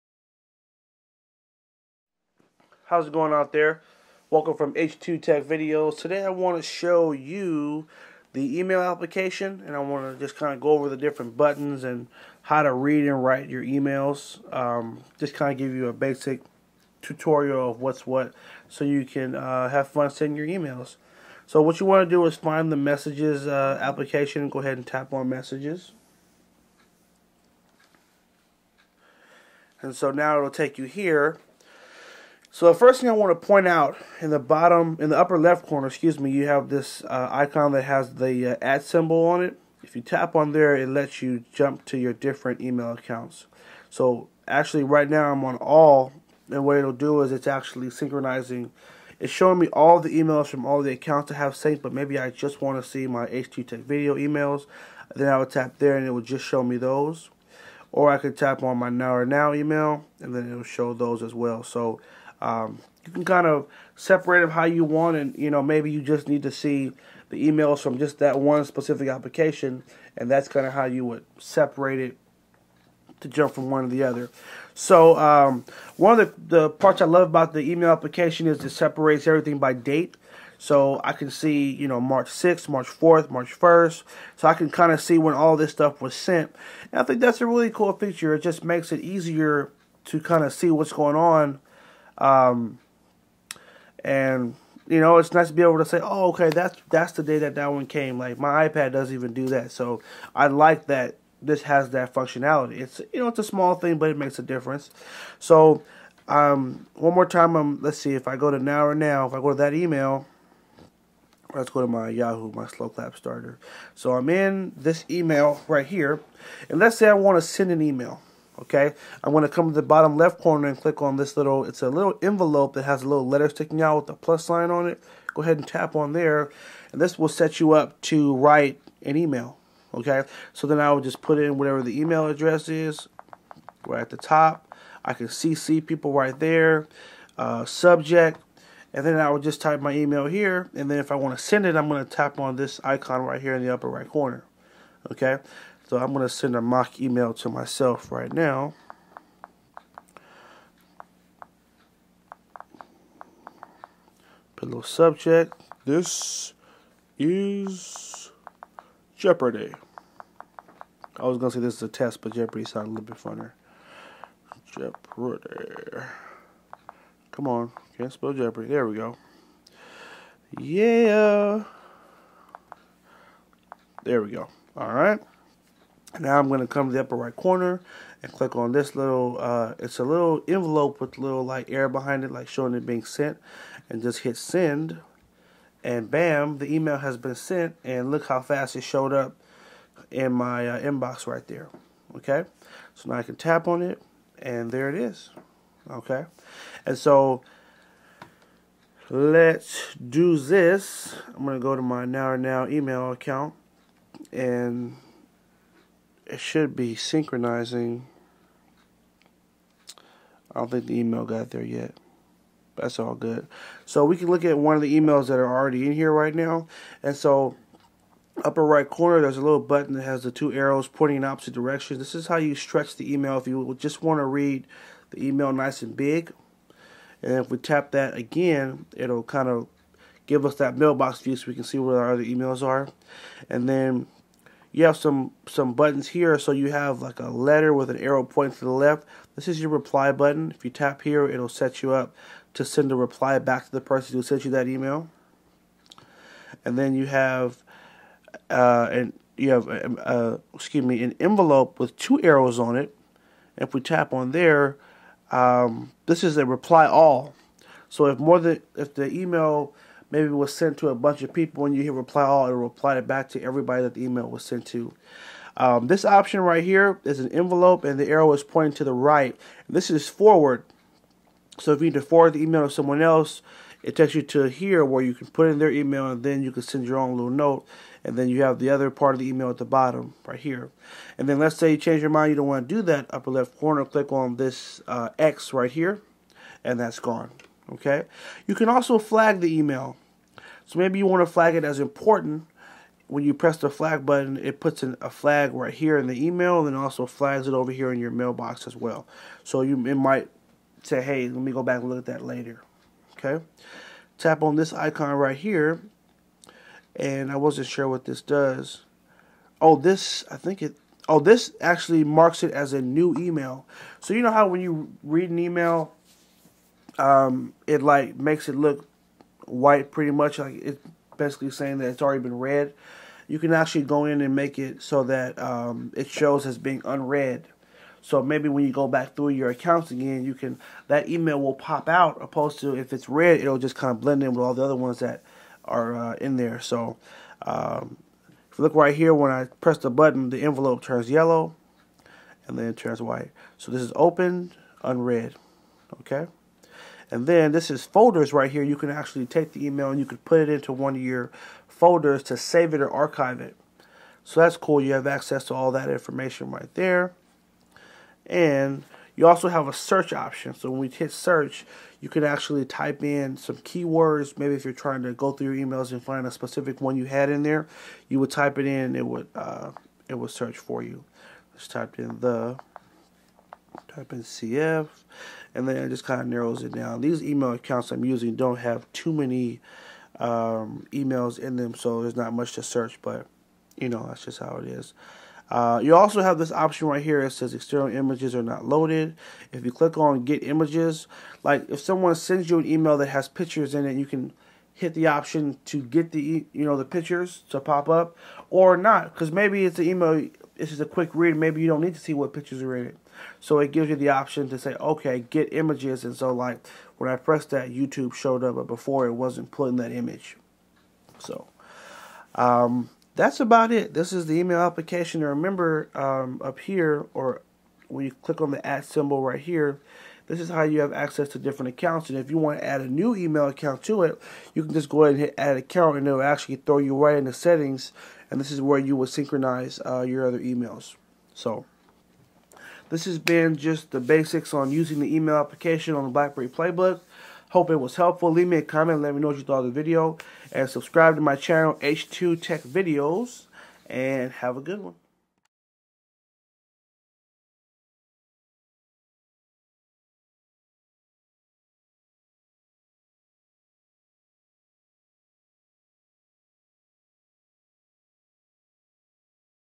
Blackberry Playbook: Sending & Receiving Emails​​​ | H2TechVideos​​​
This is a video tutorial on how to send and receive emails on the blackberry playbook.​​​